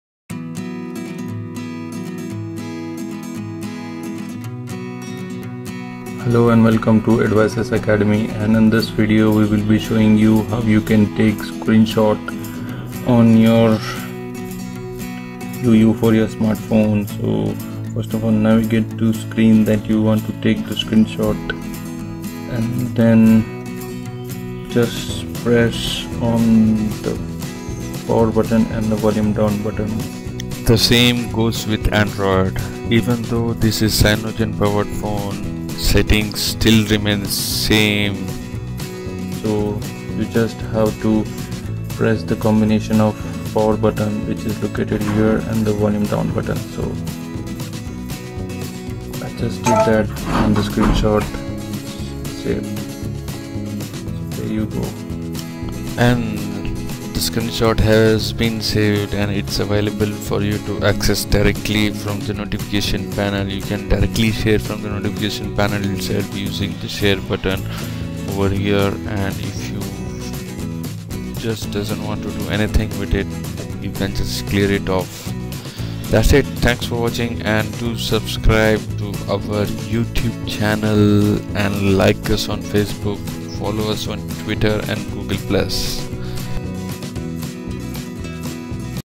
How to Take Screenshot on Yu Yuphoria Phone - Screenshot on Yu Phones
In this video checkout how to take screenshot on Micromax Yu Phones[video shows it on Yu Yuphoria YU5010 phone but the process is same for all Yu phones which includes Yu Yureka, Yureka Plus, Yu Yunique and Yu Yuphoria].